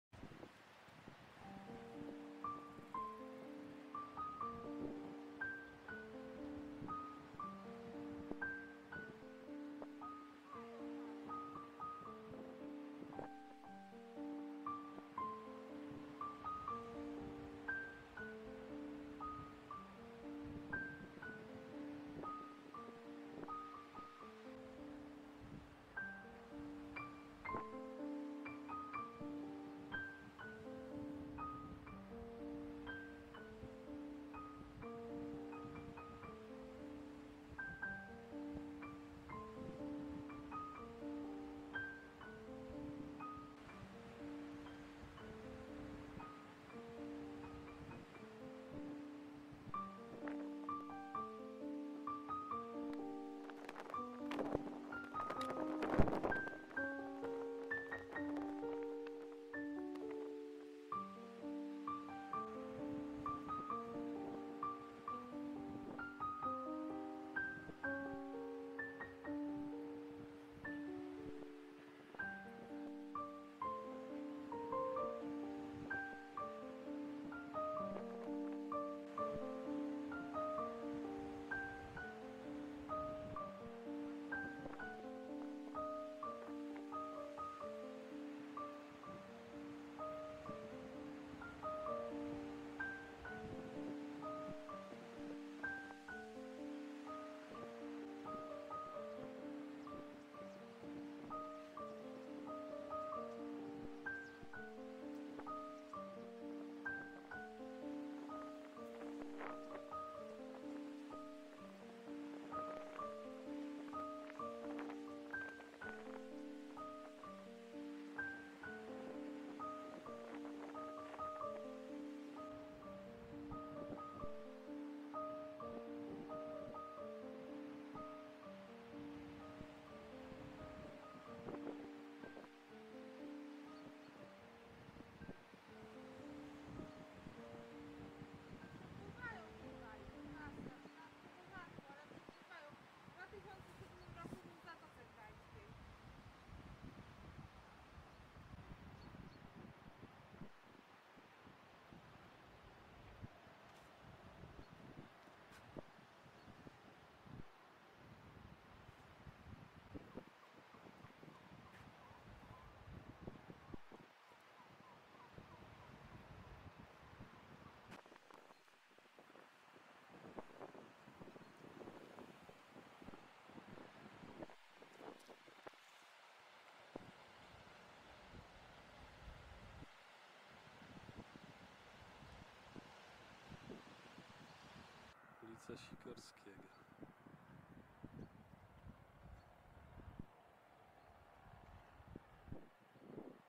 Hel i wieloryb - historia obrazkowa. Hel and the whale
Historia martwego finwala z sierpnia 2015 r. Walenia odkryto w Stegnie, skąd trafił na Hel, gdzie dokonano jego sekcji. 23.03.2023 The story of the dead fin whale from August 2015. The cetacean was discovered in Stegna, from where it was transported to Hel, where it was dissected.